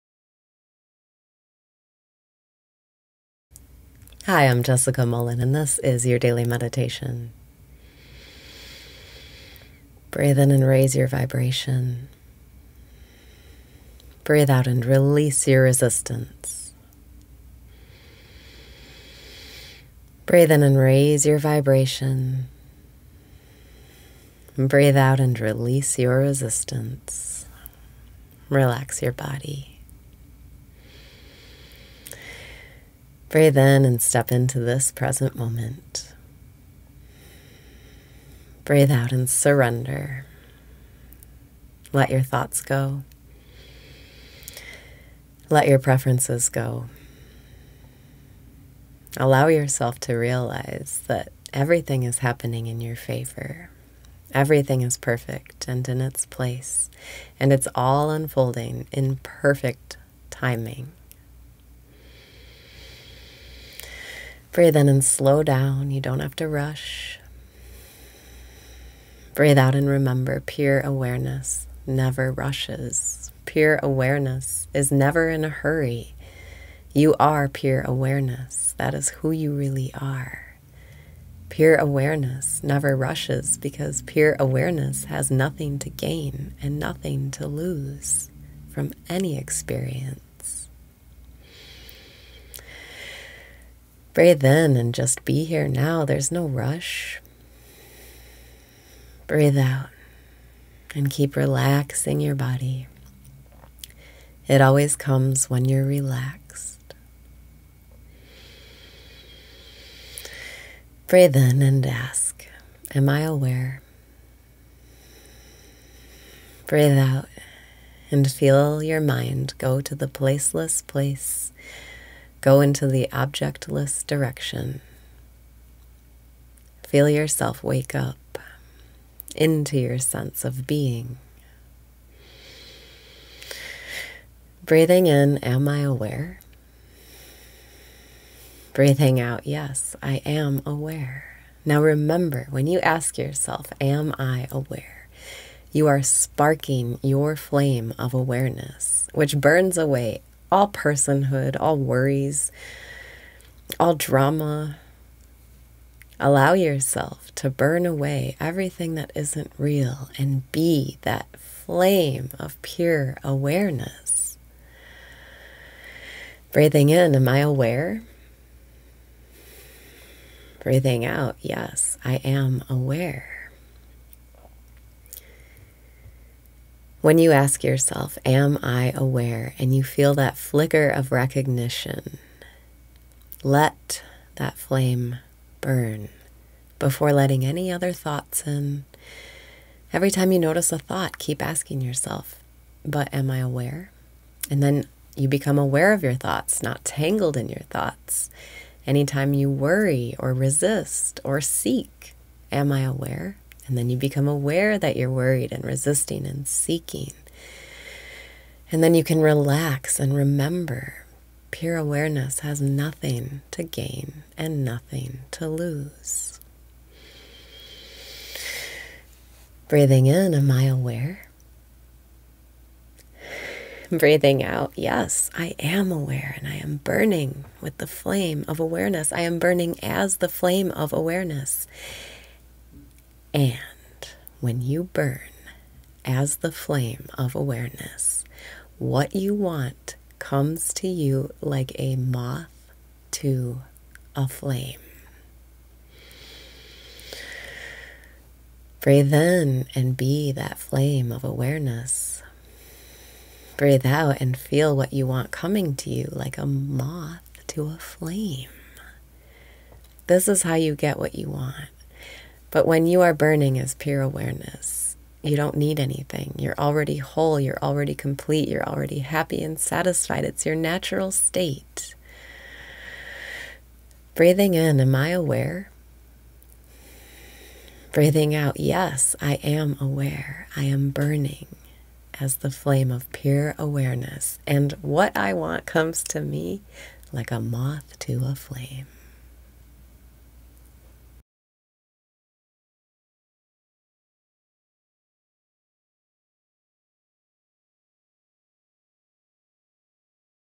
Daily Meditation - Like a Moth to a Flame
+TRANSCRIPT+

Breathe in and step into this present moment.
Breathe out and surrender. Let your thoughts go.

Let your preferences go. Allow yourself to realize that everything is happening in your favor. Everything is perfect and in its place. And it’s all unfolding in perfect timing.

Breathe in and slow down. You don’t have to rush.
Breathe out and remember: pure awareness never rushes.

Pure awareness is never in a hurry. You are pure awareness. That is who you really are. Pure awareness never rushes because pure awareness has nothing to gain and nothing to lose from any experience!

Breathe in and just be here now. There’s no rush.
Breathe out and keep relaxing your body.

It always comes when you’re relaxed.

Breathe out and feel your mind go to the placeless place. Go into the objectless direction.

Feel yourself wake up into your sense of being.

Now remember when you ask yourself, “Am I aware?” you are sparking your flame of awareness, which burns away all personhood, all worries, all drama. Allow yourself to burn away everything that isn’t real, and be that flame of pure awareness!

When you ask yourself, “Am I aware?” And you feel that flicker of recognition, let that flame burn before letting any other thoughts in. Every time you notice a thought, keep asking yourself, “But am I aware?” And then you become aware of your thoughts, not tangled in your thoughts. Any time you worry or resist or seek, “Am I aware?” And then you become aware that you’re worried and resisting and seeking. And then you can relax and remember: pure awareness has nothing to gain and nothing to lose.

Breathing out, yes I am aware! I am burning with the flame of awareness!

I am burning as the flame of awareness! And when you burn as the flame of awareness, what you want comes to you, like a moth to a flame.

Breathe in and be that flame of awareness.
Breathe out and feel what you want coming to you, like a moth to a flame!

This is how you get what you want. But when you are burning as pure awareness, you don’t need anything. You’re already whole, you’re already complete, you’re already happy and satisfied. It’s your natural state.

I am burning as the flame of pure awareness. And what I want comes to me like a moth to a flame!